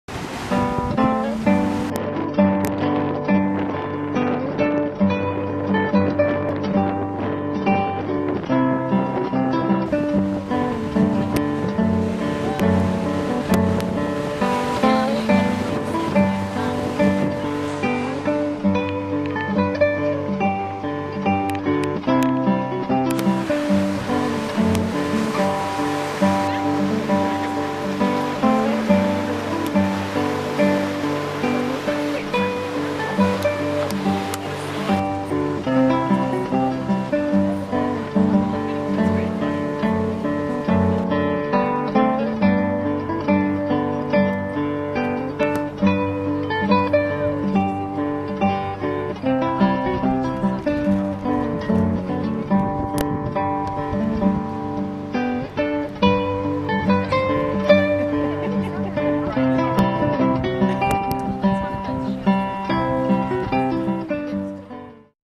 2016 Sculptures by the Sea in Sydney
Staged on the spectacular Bondi to Tamarama coastal walk, Sculpture by the Sea, Bondi is one of Sydney’s most popular events, with 520,000 visitors viewing over 100 sculptures by artists from around the world.